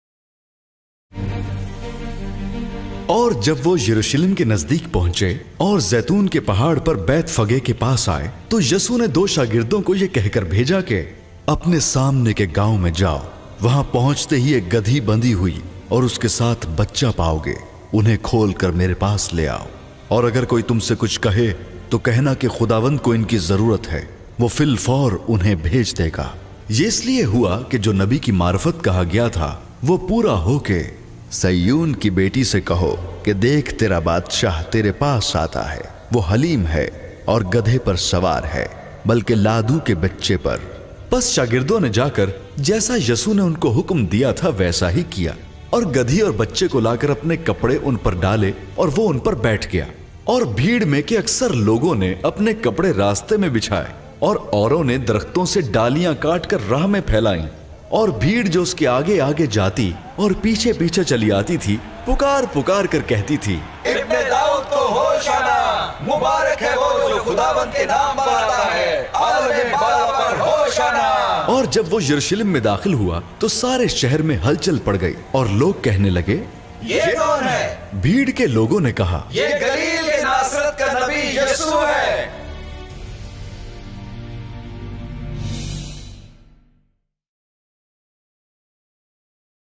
078 - شاہانہ داخلہ (Urdu-Pakistani)
کلام کا حوالہ: متّی21: 1-11
یسوع گدھے پر سوار ہو کر یروشلیم میں داخل ہوا، اور لوگ"ہوشعنا‘‘ پکارتے ہیں"

سوال 1: زمینی بادشاہت اور آسمانی بادشاہت کے قیام میں کیا عملی فرق ہے؟
آپ واقعی کس مملکت کا حصہ بننا چاہتے ہیں؟ کیوں؟

سوال 2: اگر ہم یسوع کو بادشاہ کے طور پر دیکھتے ہیں جیسا کہ ہم اسے نجات دہندہ کے طور پر د
یکھتے ہیں تو یہ ہمارے نقطہ نظر کو کیسے بدلے گا کہ مسیحی ہونے کا کیا مطلب ہے؟

سوال 3: اگر آپ یسوع کو شہر میں سوار ہوتے دیکھنے کے لیے وہاں موجود ہوتے تو آپ کا کیا ردعمل ہوتا؟

گلوبل گوسپل  گفتگو کے سوالات دیکھیں، ڈاؤن لوڈ   یا   پرنٹ کریں۔